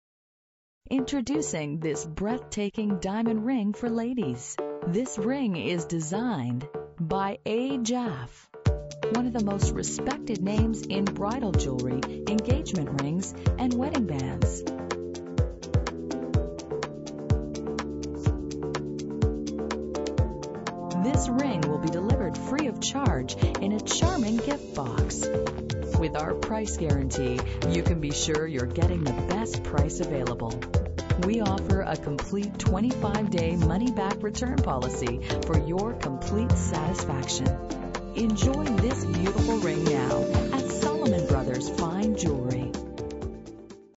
A. Jaffe ME1572/40 Engagement Rings
A. Jaffe ME1572/40 28 Stone Round Shared prong Set 2 Row Diamond Engagement Ring with Bezel Accents Diamond .39cttw Gh/vs2 is brought to you , The best online source for fine jewelry, Engagement Rings and diamonds.